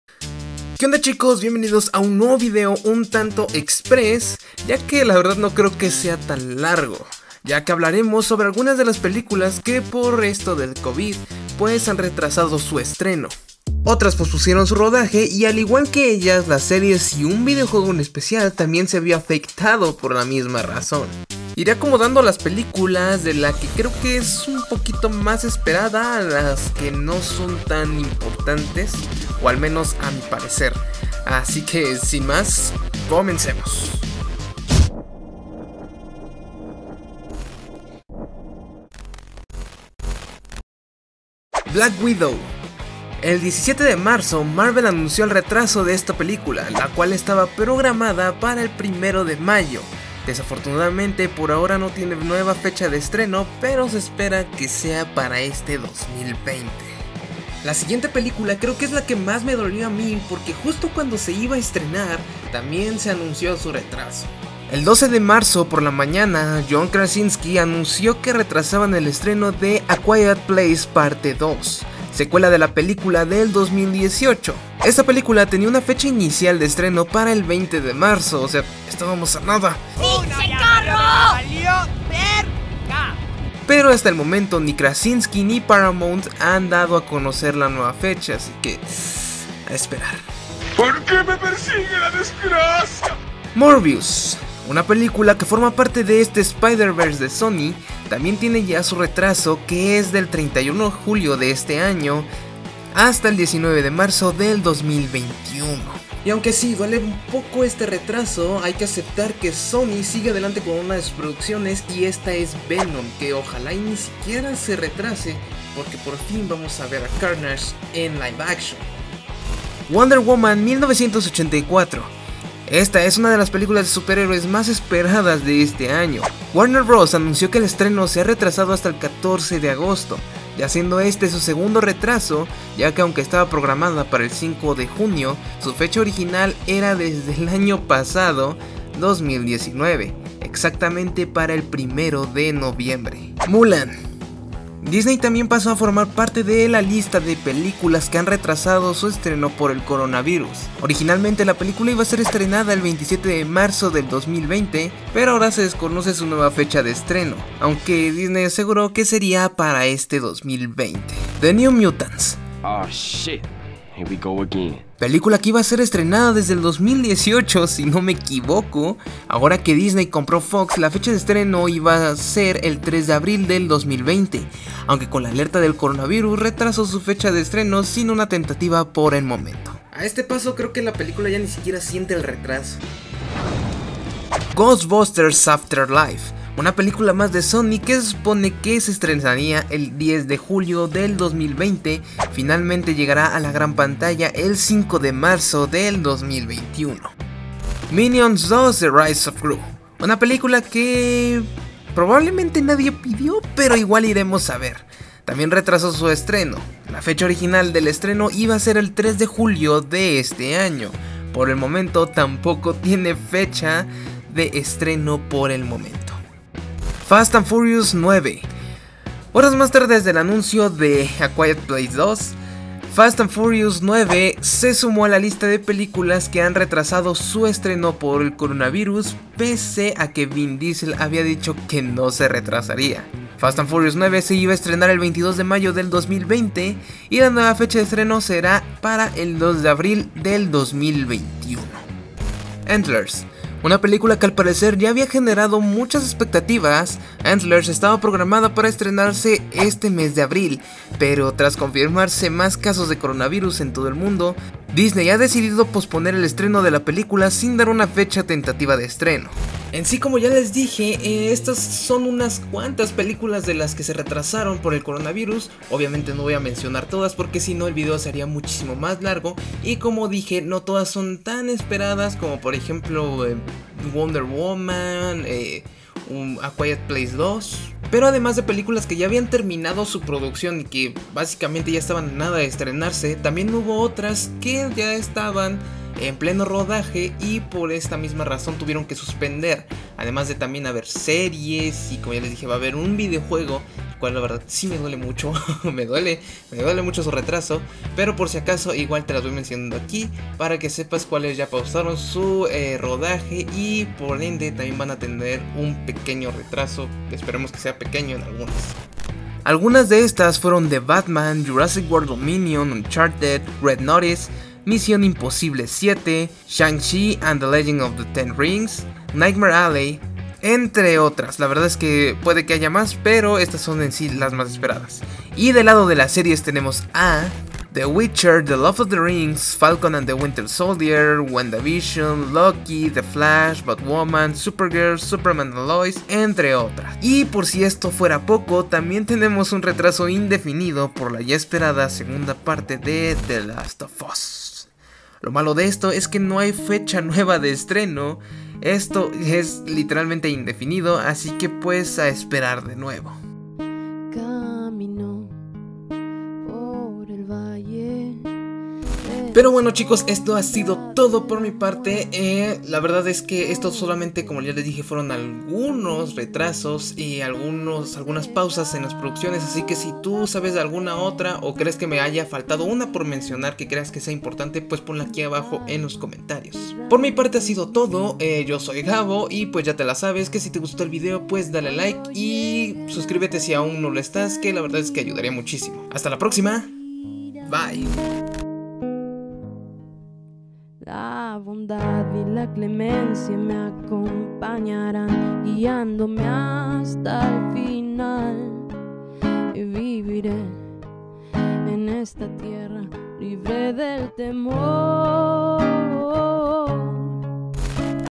Retrasos en Cine, Series y Videojuegos del 2020!!!
Que onda chicos!!! vienvenidos a un nuevi video en el cual les presento algunos de los retrasos mas importantes de este año 2020 provocados a la pandemia actual!

Espero y les guste y me puedan dejar su like y su suscripción!!!

Muchas gracias Bye!!!

ID (PS4): GaboGeek